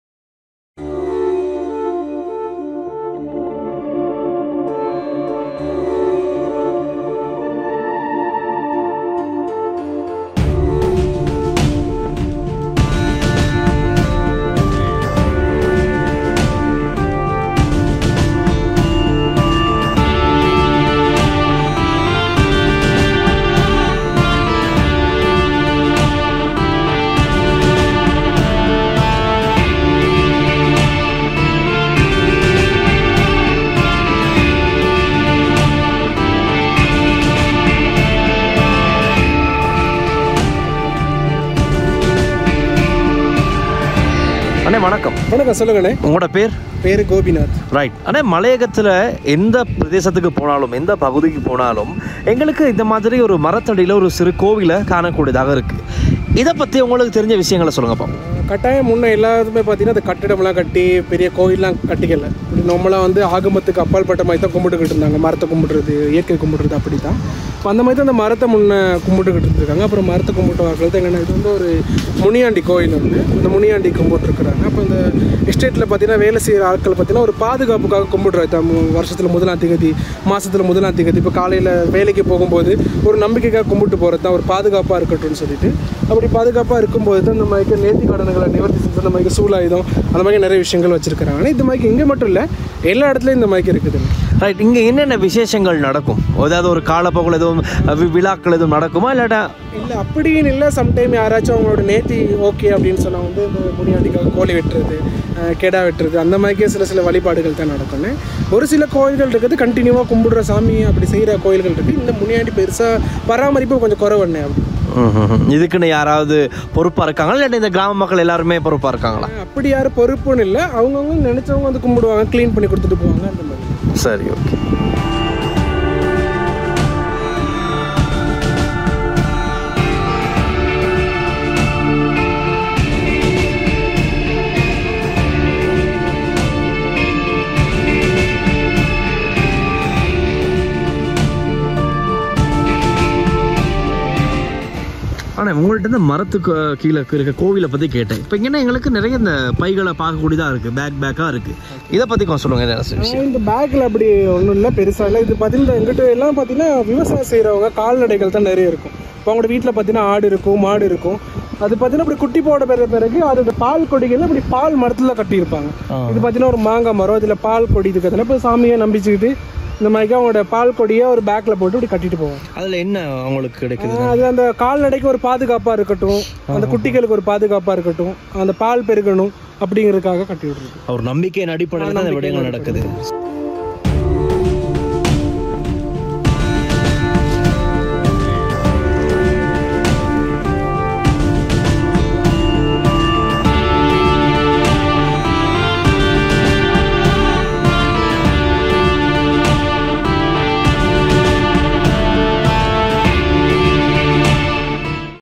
ஊரையே காக்கும் அதிசய மரம்! வேண்டியதை கொடுக்கும் தெய்வம்! இதன் பின்னணி என்ன?
பெருந்தோட்ட பகுதிகளில் பல இடங்களில் மரங்களை அல்லது அதன் அடியில் இருக்கும் தெய்வங்களை வழிபடும் வழக்கம் இன்று வரை காணப்படுகிறது.

குறிப்பாக பெரிய மரங்களின் கீழ் சிலைகளை வைத்து வழிபடுவது தொன்றுதொட்டு வரும் வழக்கமாக இருக்கிறது.

அந்த வகையில் அவ்வாறானதொரு மரம் தொடர்பில் கீழ்வரும் காணொளி விவரிக்கிறது.